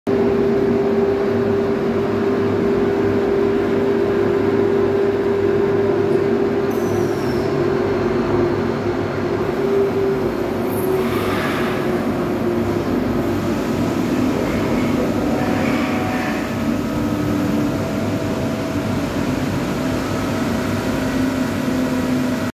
Multitech - Videojet 2120 Large Character Inkjet Marking
The Videojet 2120 is an easy-to-use printer for applying single or twin line alphanumeric codes to a variety of products or packages. A choice of printheads and inks enable it to print on a wide range of porous and non-porous materials. The 2120 can control two printheads for opposite side case coding, and also offers multiple printer control from one screen for more complex, multi-line coding.